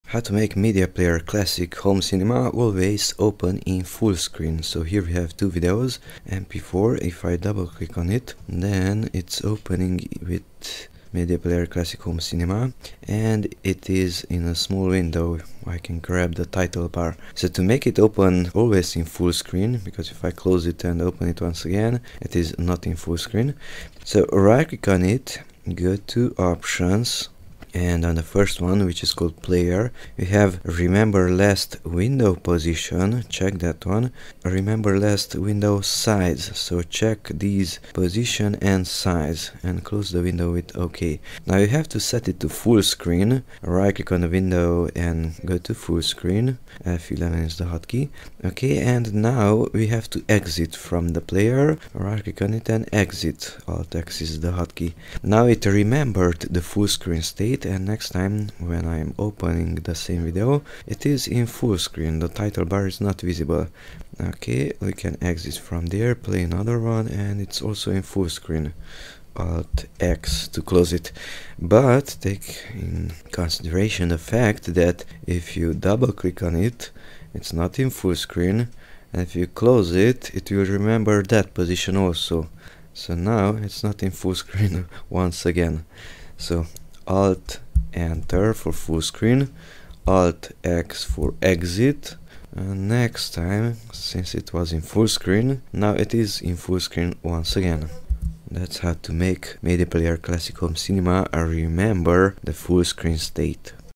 How to make MPC-HC start in Full Screen all the time?
- Options (shortcut key = O)
- Player

Check these:
- Remember last window position
- Remember last window size

- Alt + Enter for Fullscreen
- Alt + X to close